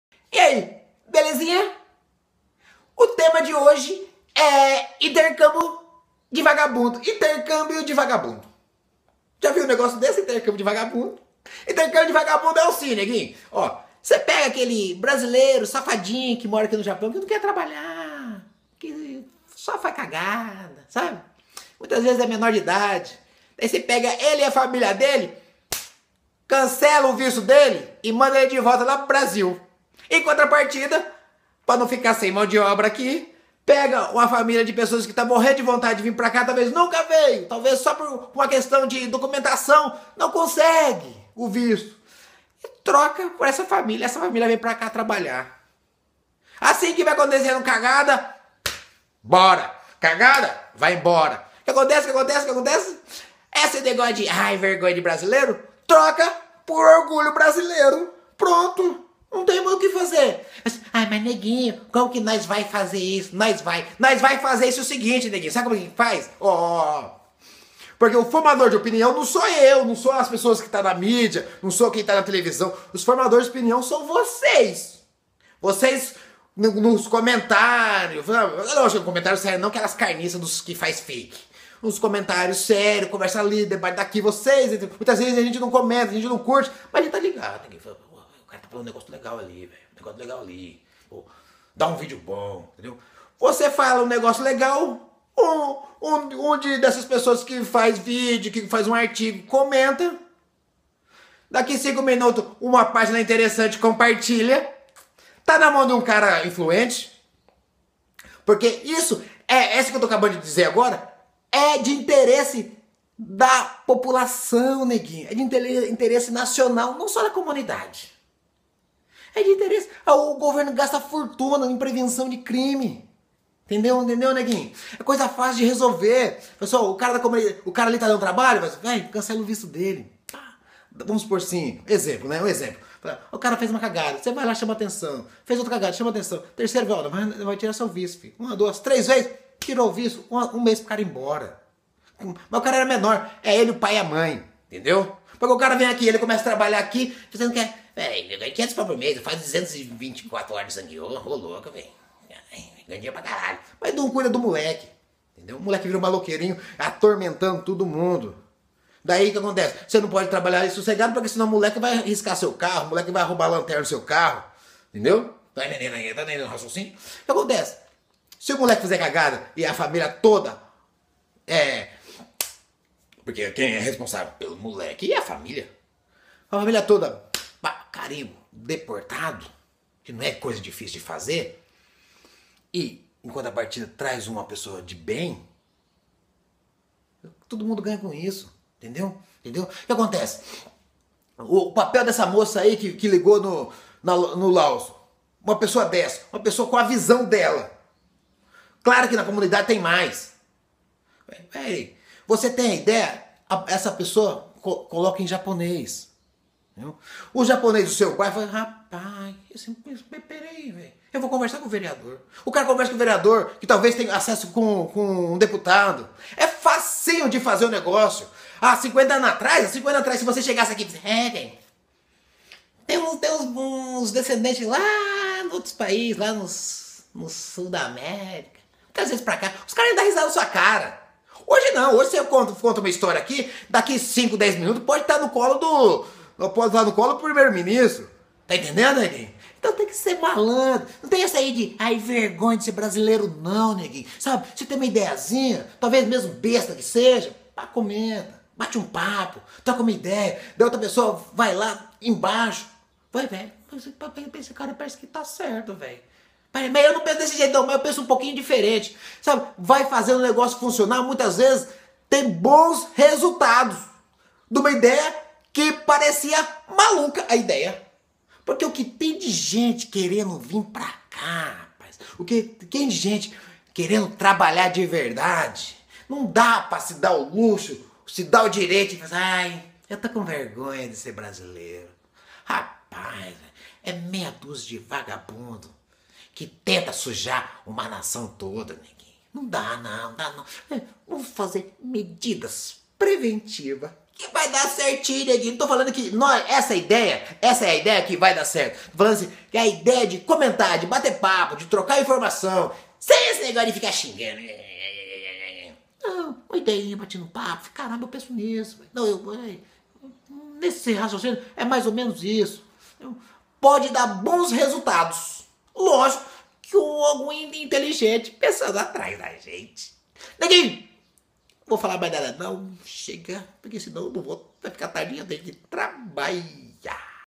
Intercambio de vagabundo no JAPAO.
Tem tantas Brasileiros no Brasil querendo uma chance, para morar no Japão. E alguns aqui, fazendo cagadas.